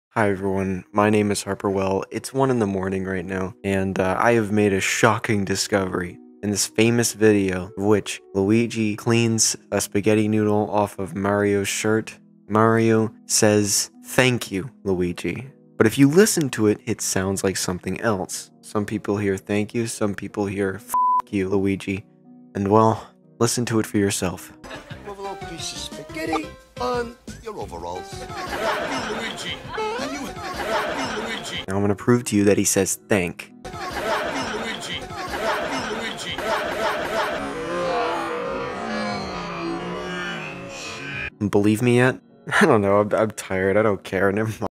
he says thank you...
so i JUST now realized mario ACTUALLY says 'thank you luigi' after years of believing otherwise. because i am STUPID .

facade vid coming soon, as well as some other things, but i figured this would be something at least